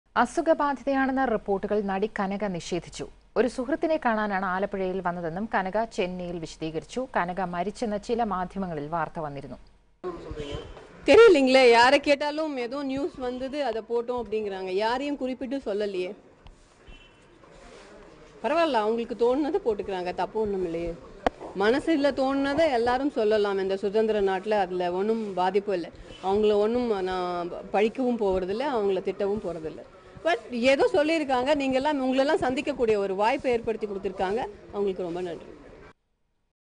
Actress Kanaka still alive കനക
Kanaka appeared in public and made it clear that she was alive